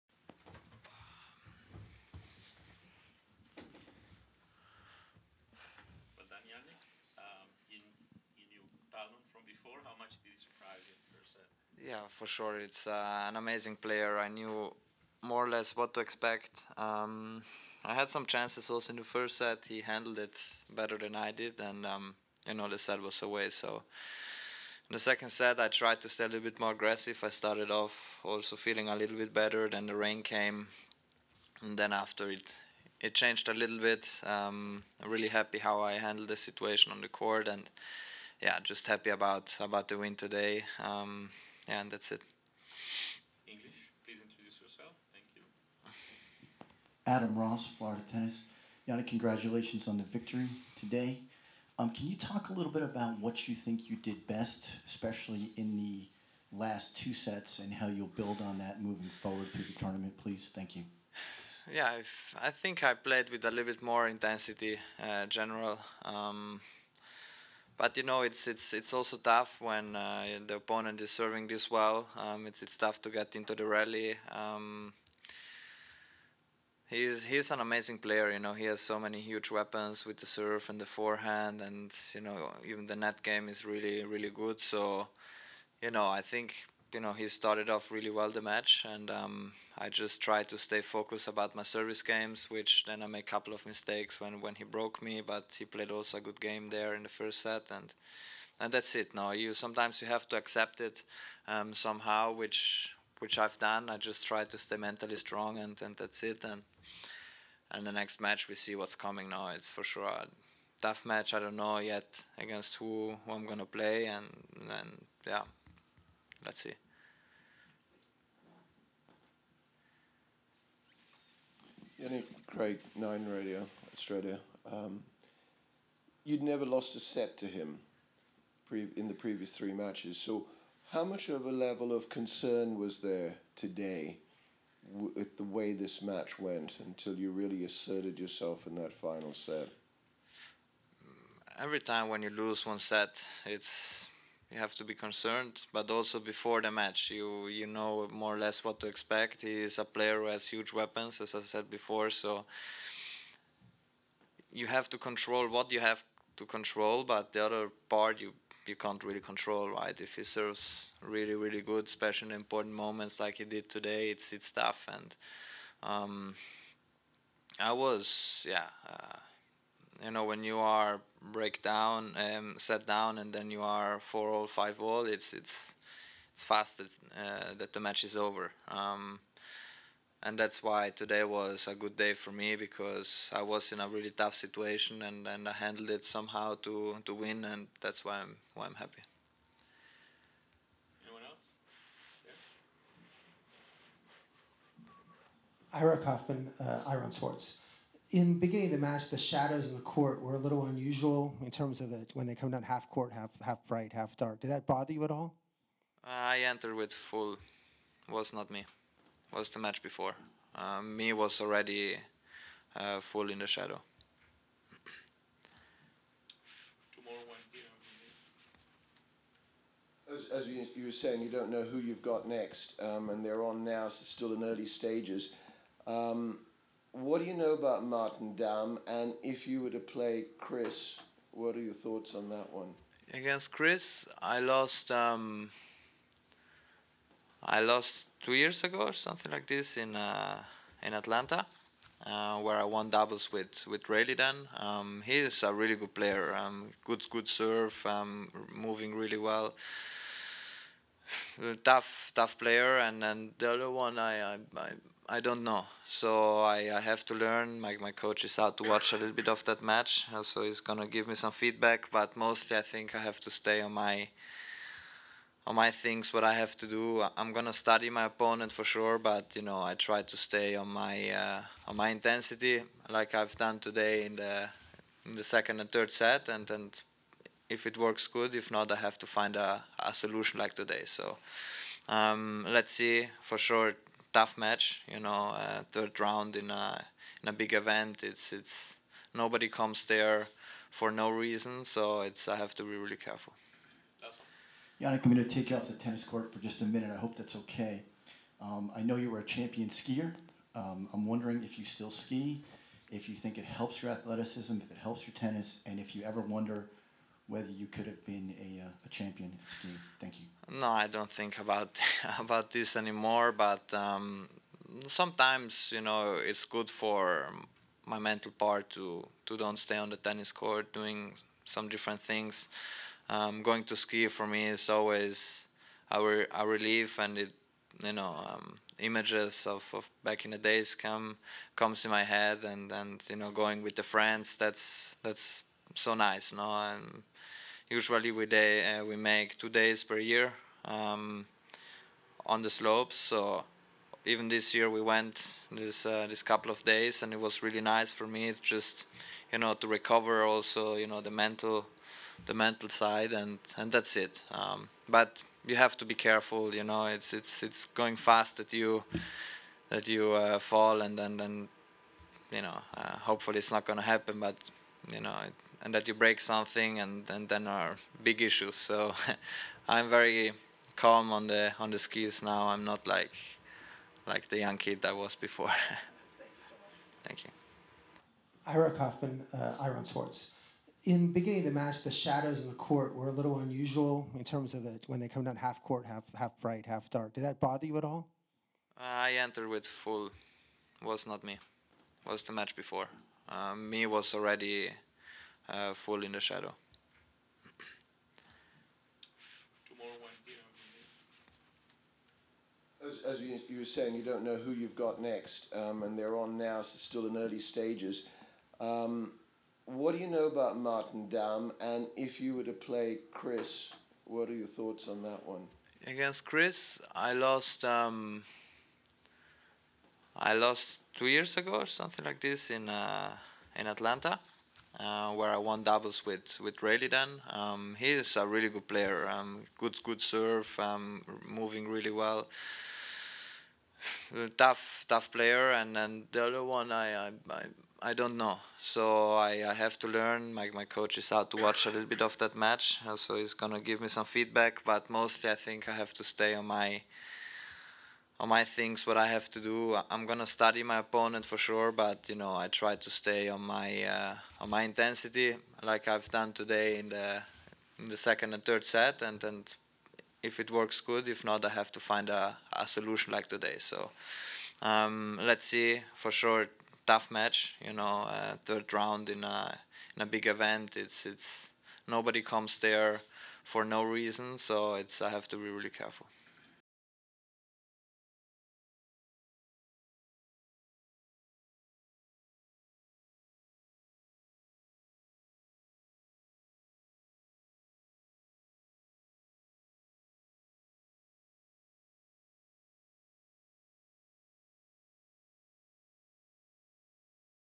Jannik Sinner "I'm not like the young kid I was before" - Miami 2024
Jannik Sinner Press Conference After Match vs Griekspoor - Miami 2024